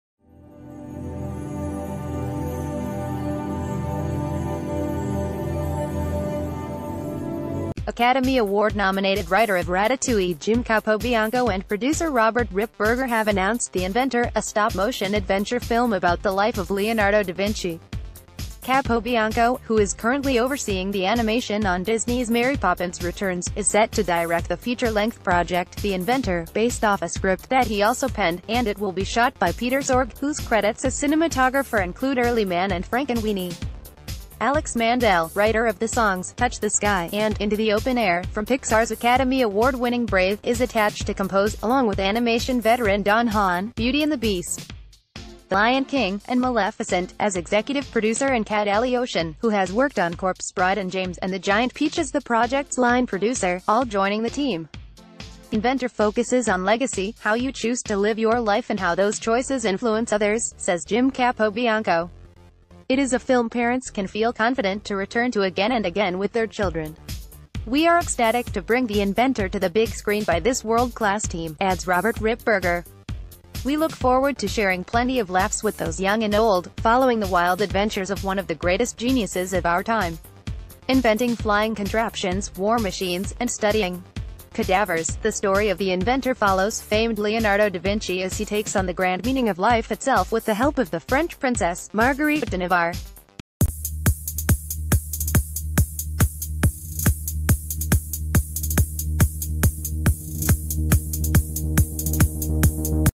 Breaking News | Stop-motion Leonardo Da Vinci movie The Inventor in development
Academy Award nominated writer of Ratatouille Jim Capobianco and producer Robert Rippberger have announced The Inventor, a stop-motion adventure film about the life of Leonardo da Vinci. Capobianco, who is currently overseeing the animation on Disney’s Mary Poppins Returns, is set to direct the feature-length project, The Inventor, based off a script that he also penned, and it will be shot by Peter Sorg, whose credits as cinematographer include Early Man and Frankenweenie. Alex Mandel, writer o...